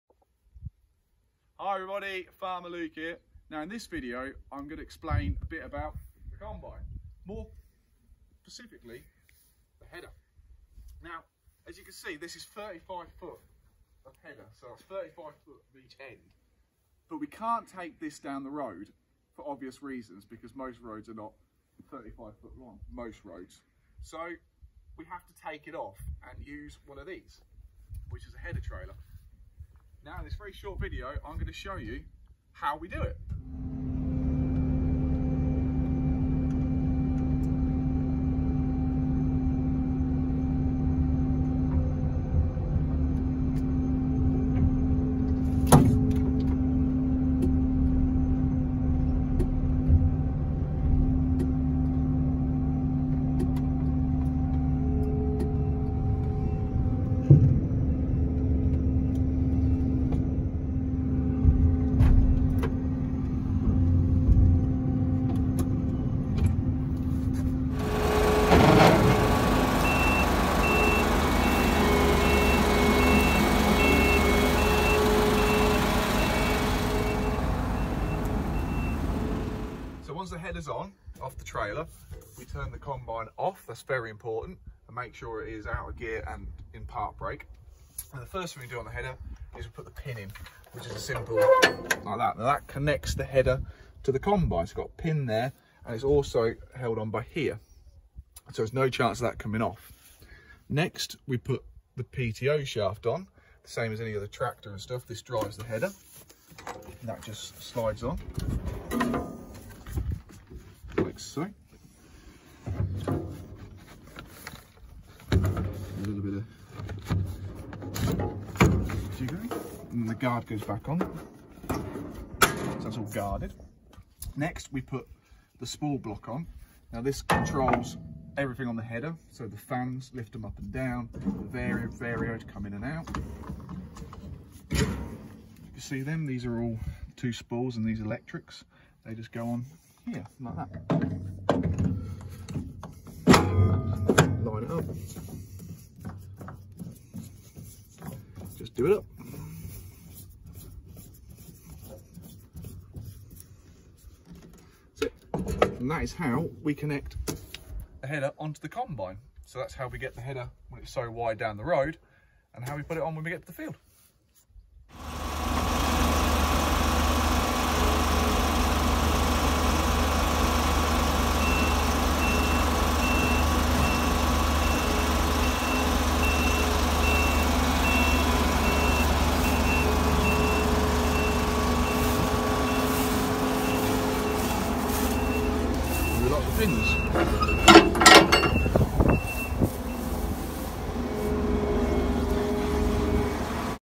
How do farmers get those massive combine headers/cutter bars down the road safely? 🚜🌾
How do farmers get those massive combine headers/cutter bars down the road safely? Farmer Luke explains🚜🌾